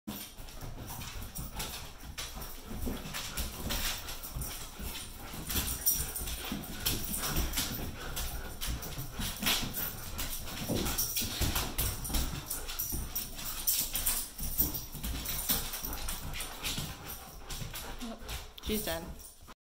Ace showing Aussie puppy how to play.
Puppy flashback: Ace, German shepherd, showing River, Australian shepherd, how to play when she was a puppy.